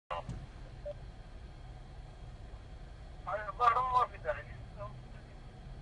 Car bursts into flames on Umm Suqeim road in Dubai
A vehicle burst into flames on Umm Suqeim road near the Al Khail exit in Dubai on Sunday.
"Thankfully, no was was killed or injured, and the Dubai Civil Defense was able to almost immediately put out the fire," said Col Al Mazrouei.

Video courtesy Dubai Police